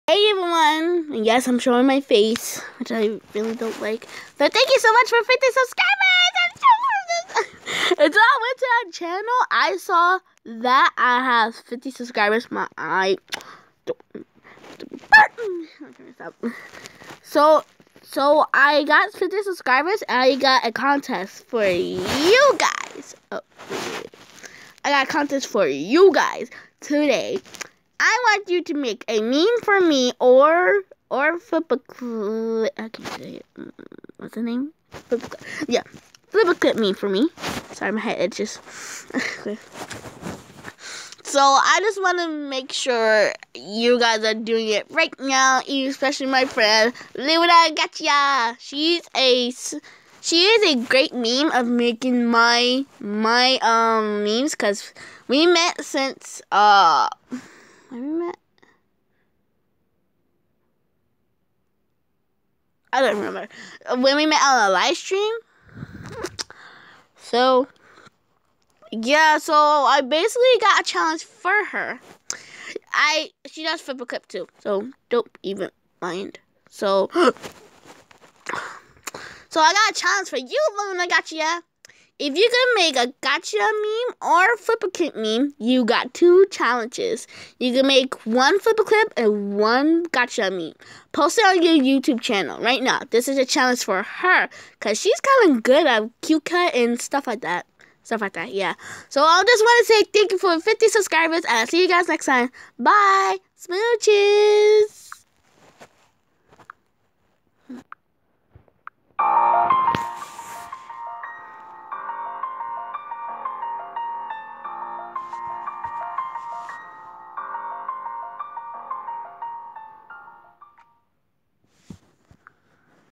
A CHALLENGE FOR MY BFFLUNAGACHA 50 SUBS
Luna I got a challenge for u the challenge:I want u to make a Flipaclip meme for me/gacha meme u can one of them good luck ;3